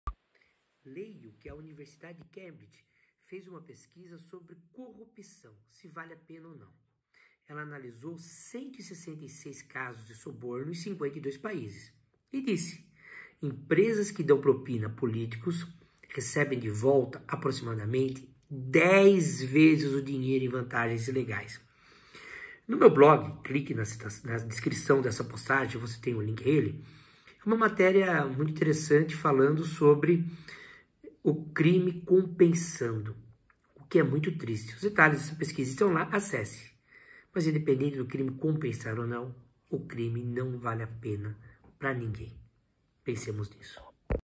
3333. Infelizmente, para muitos, a corrupção cavaleiro a pena.
CORRUPÇÃO -
Infelizmente, muitas vezes o crime compensa (para quem não tem escrúpulos).